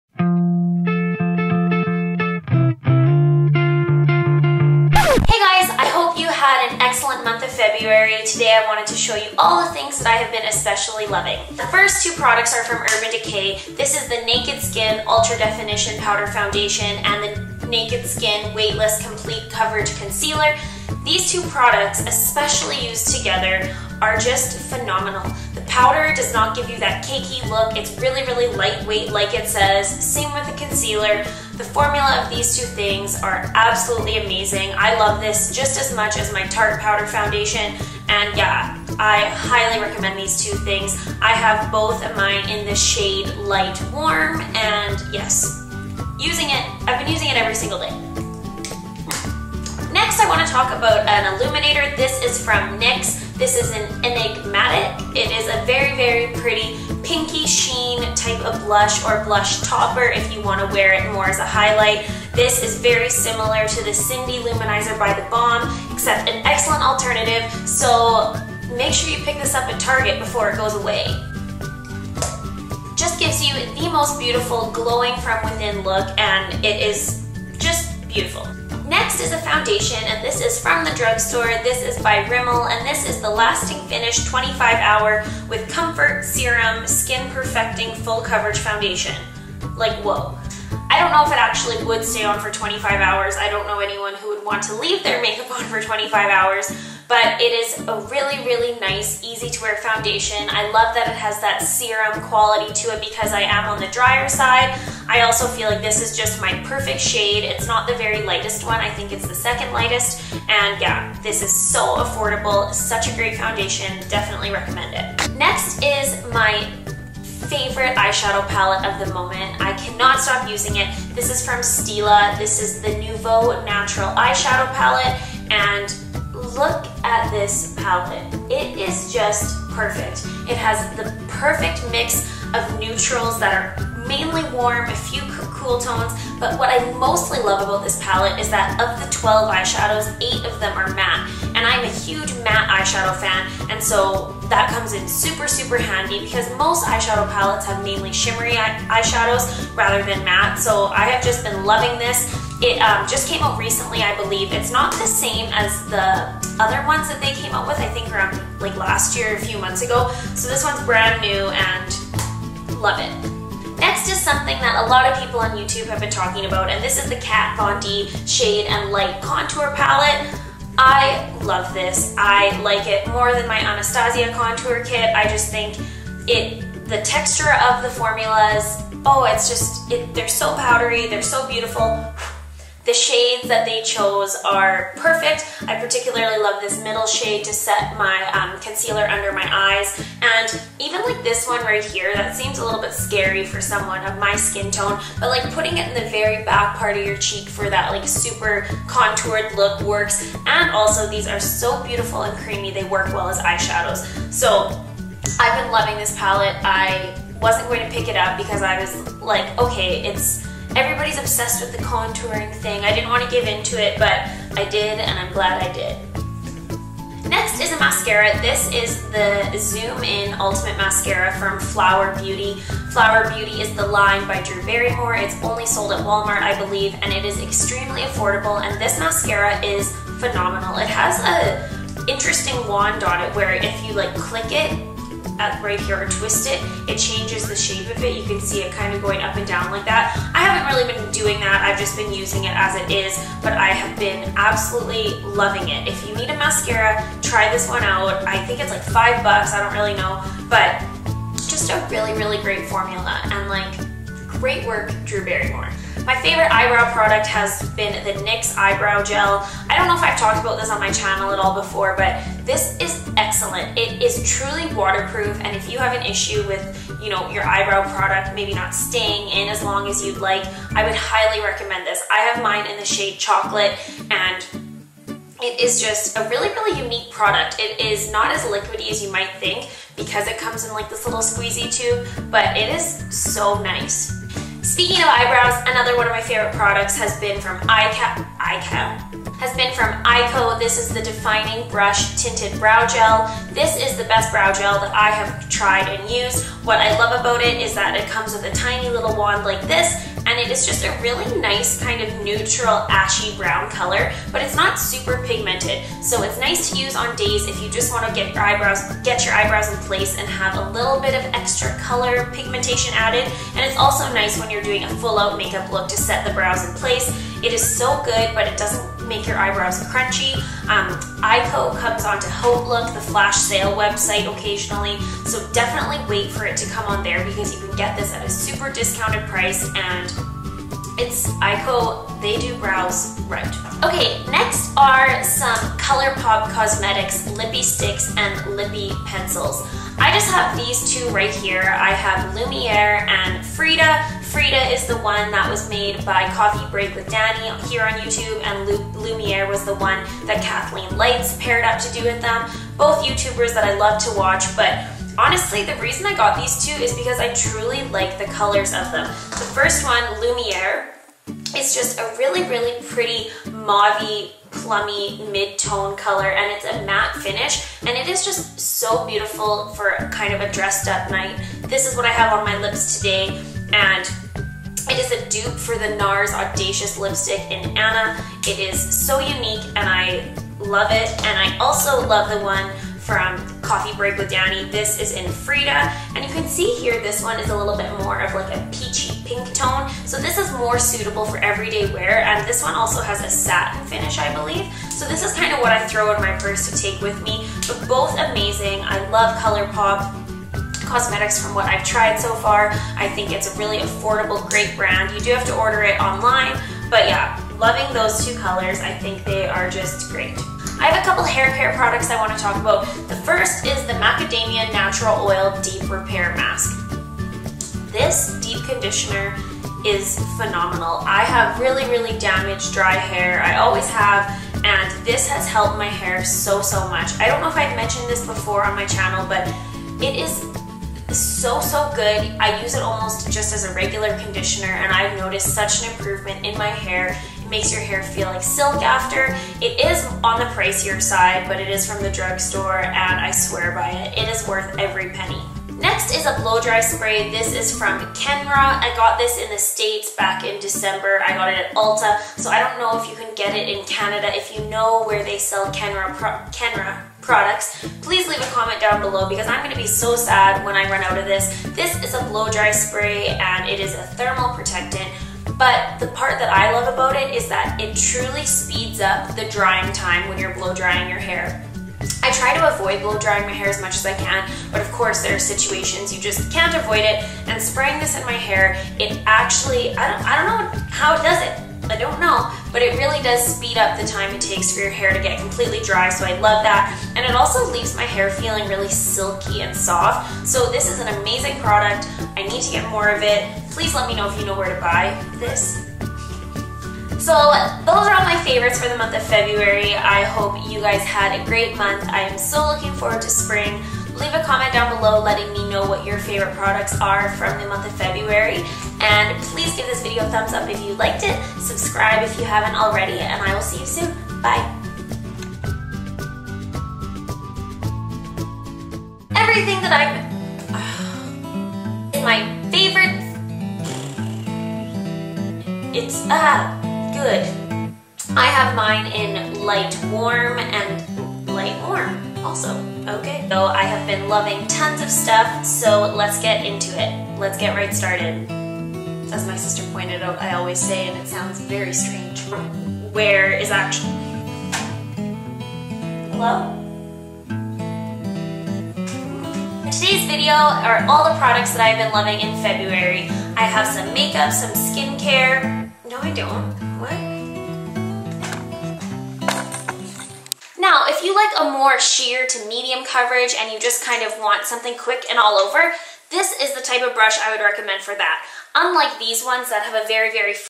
February Favorites 2015 | Urban Decay, Stila, NYX + more!
Thumbs up for the month of LOVE!

Monthly Faves Time!

Products Mentioned:
Urban Decay Naked Skin Ultra Definition Powder Foundation
Urban Decay Naked Skin Weightless Complete Coverage Concealer
NYX Illuminator - Enigmatic
Rimmel Lasting Finish 25 Hour Comfort Serum Foundation - 100 Ivory
Stila Nouveau Natural Eye Shadow Palette
Kat Von D Shade & Light Contour Palette
Flower Beauty Zoom-In Ultimate Mascara
NYX Waterproof Eyebrow Gel - Chocolate
Colourpop Lippie Sticks - Lumiere & Frida
Macadamia Natural Oil Deep Repair Masque
Kenra Blow Dry Spray

Hope you are having a wonderful day wherever you may be! xo